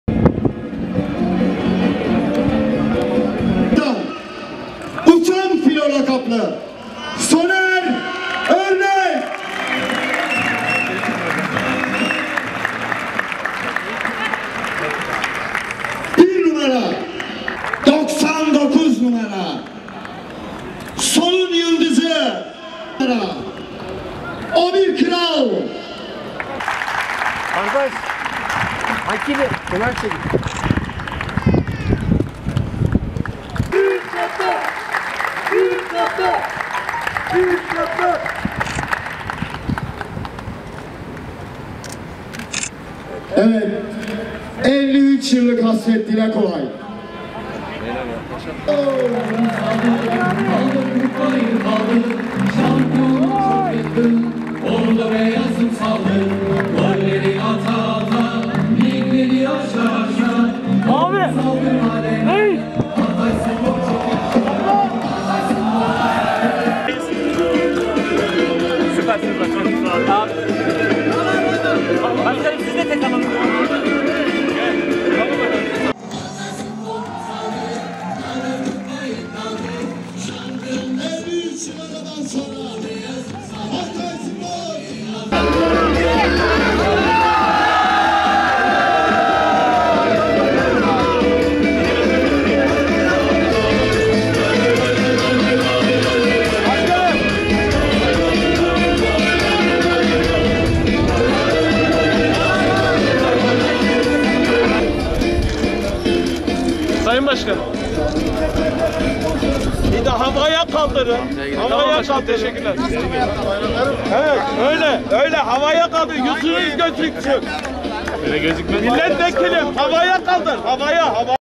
Hatayspor'da Şampiyonluk Kupası coşkusu...
TFF 1. Lig’de şampiyon olarak Süper Lig’e yükselerek 53 yıllık özleme son veren Hatayspor Federasyon Başkanı Nihat Özdemir’in de katıldığı bir törenle şampiyonluk kupasına kavuştu.